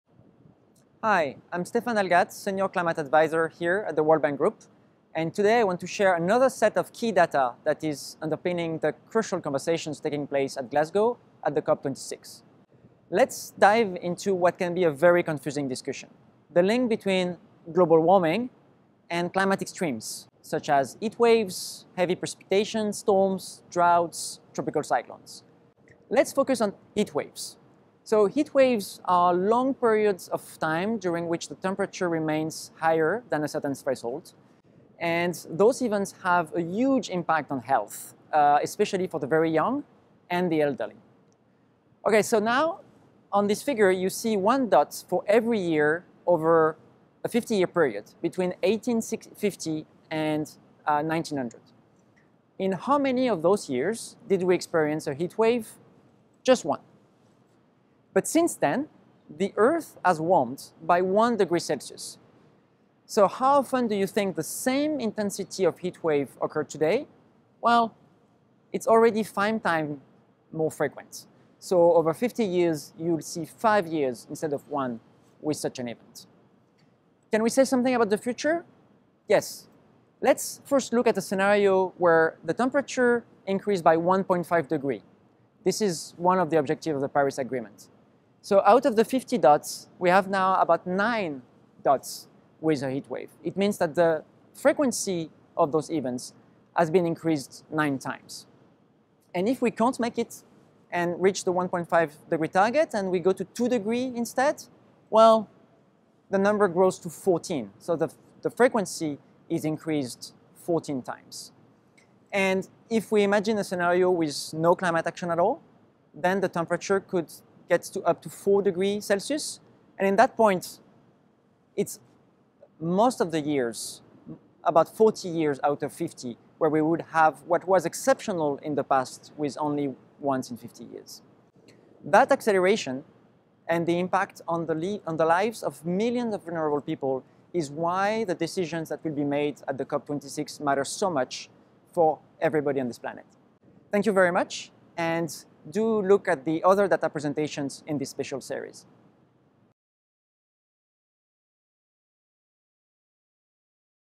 Climate Data COP26: Heatwaves Over 50 Years and Their Impact on the Lives of Vulnerable People
Heatwaves are long periods of time during which the temperature remains higher than a certain threshold. And those events have a huge impact on health‚ particularly for the very young and the elderly. Over a 50 year period, between 1850 and 1900, in how many of those years did we experience a heatwave? Just one. But since then, the earth has warmed by 1 degree Celsius. So how often do you think the same intensity of heat wave occur today? Learn more about the World Bank Group’s participation at COP26, focused in ramping up support for green, resilient, and inclusive recoveries from the COVID-19 pandemic, as well as low-carbon transitions that put people at the center.